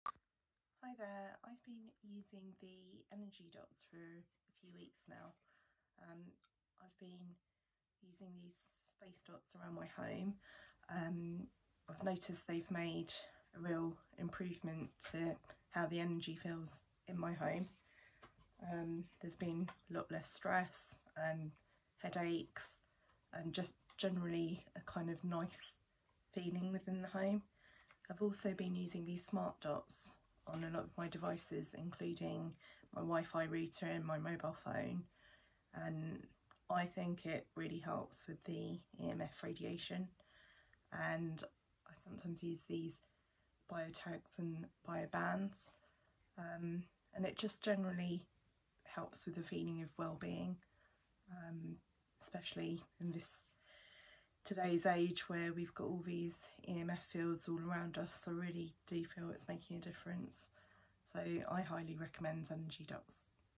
energydots Customer Review: Sheena Reviews the spaceDOT, smartDOT and bioCLIP
After trying the spaceDOT, smartDOT and bioCLIP for a couple weeks now, what Sheena has most noticed is a reduction in stress and headaches but also an improved sense of wellbeing from a calmer home environment.